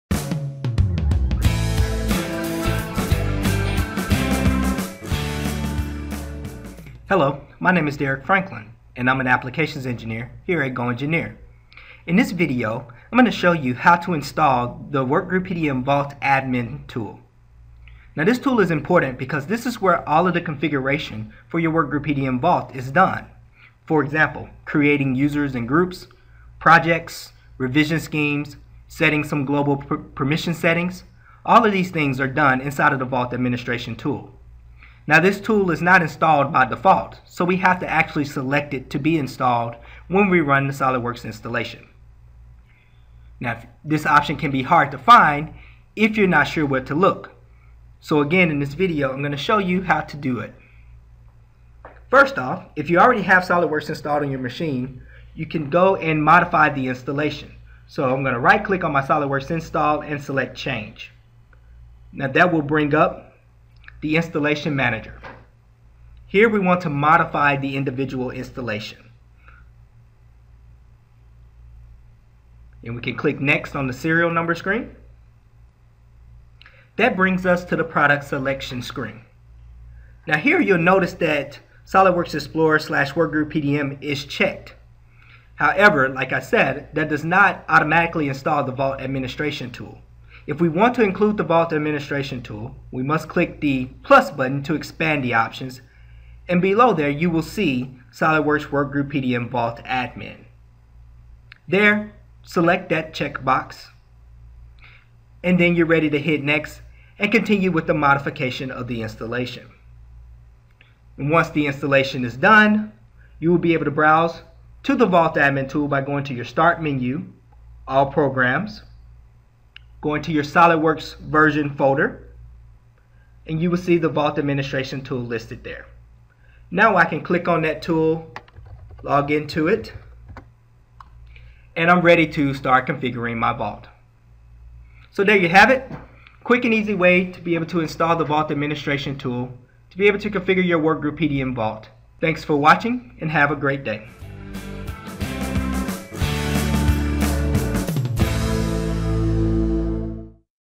SOLIDWORKS Workgroup PDM - Installing the Vault Administration Tool
Learn how to install the SOLIDWORKS Workgroup PDM Vault Administration tool for vault configuration.

Quick Tip presented by Derrick Franklin of GoEngineer.